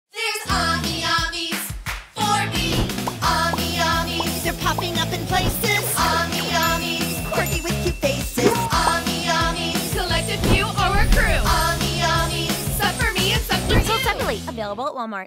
Ami Amis™ :15s TV Commercial | JAKKS Pacific
Ami Amis™ is an immersive world of cozy, knitted characters designed to delight. Our plush friends are inspired by all the joys in life. Food. Music. Animals. Nostalgia. Some friends are cute, some are quirky, and all of them are thrilled to be a part of your tight-knit crew.